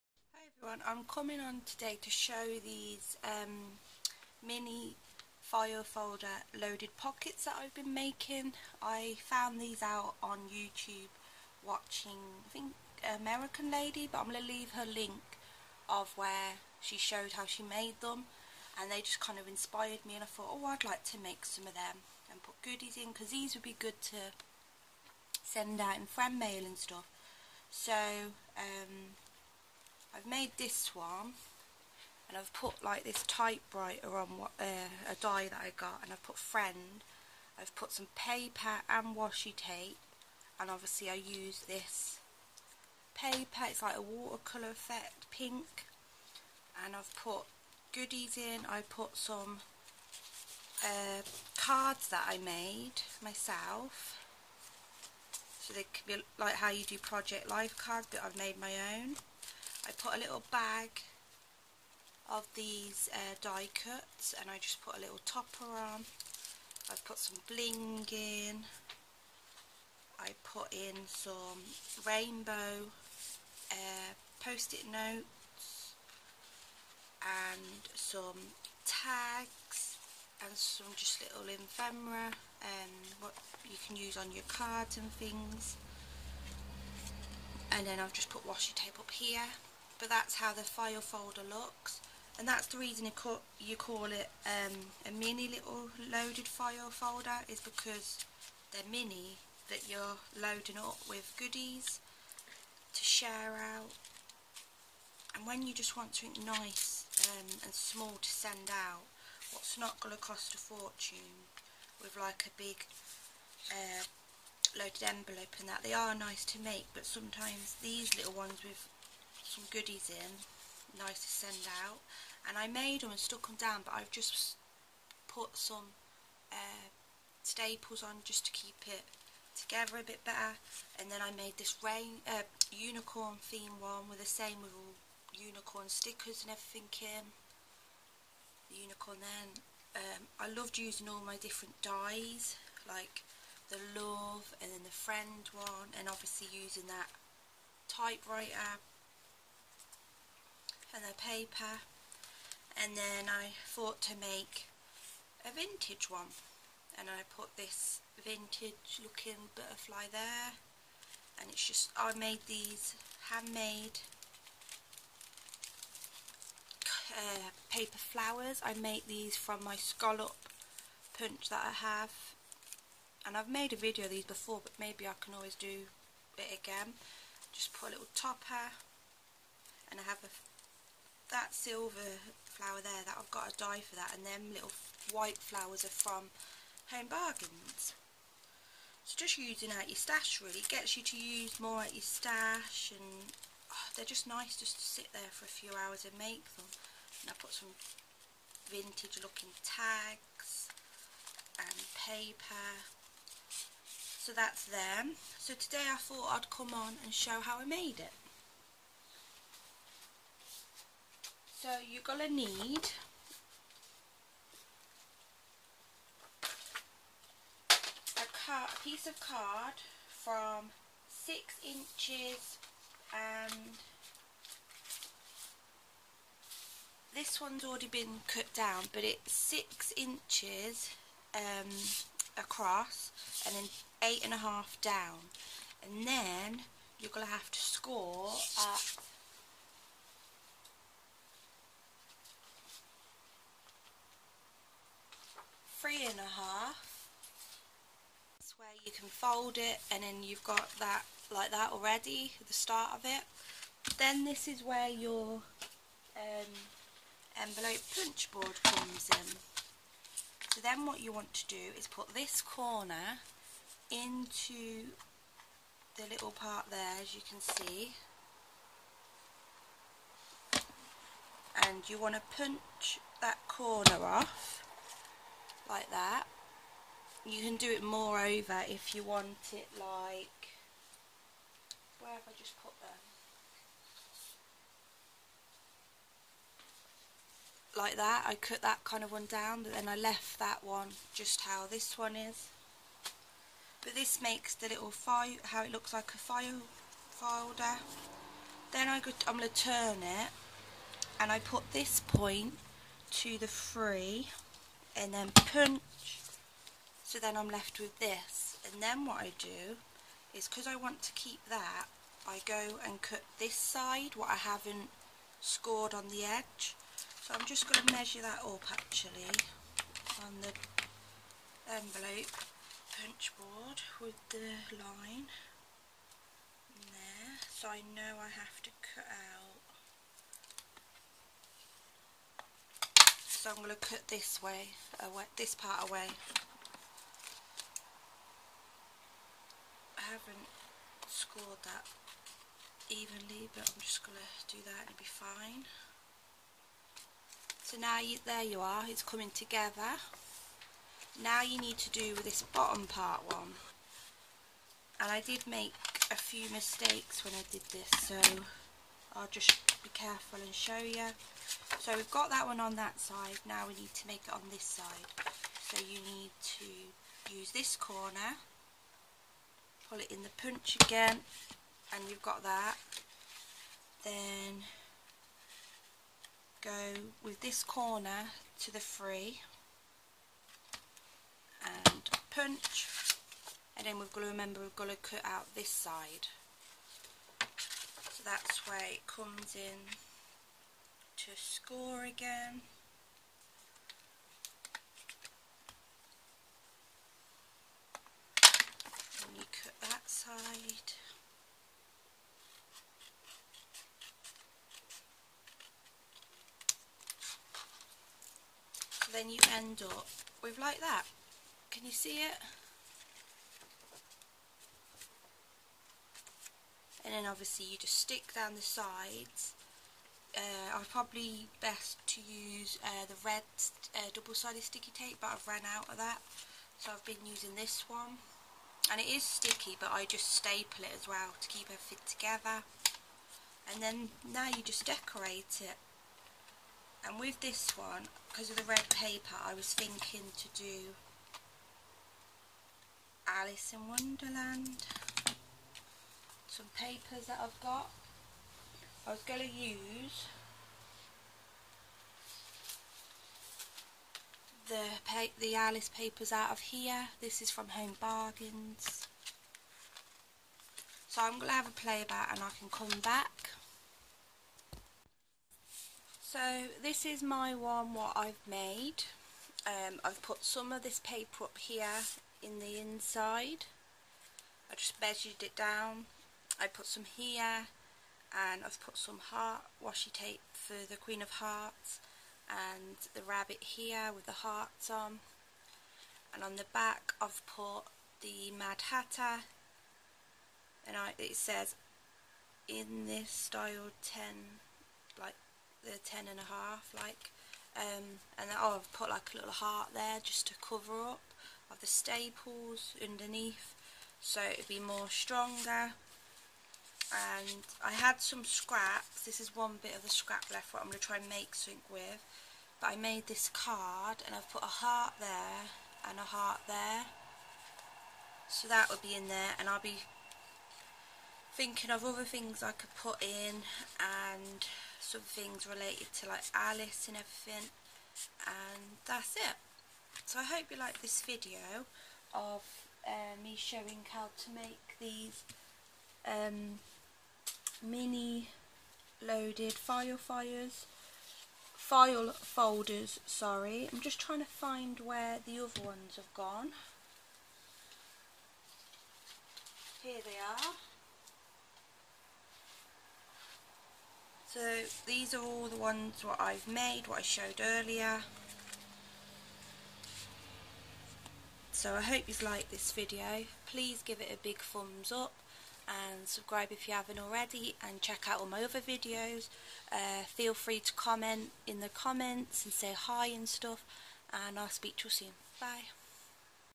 Mini loaded file Folders made on the We R Memory Keepers envelope punch board
in this video i show and make a Mini loaded file Folder made on the We R Memory Keepers envelope punch board.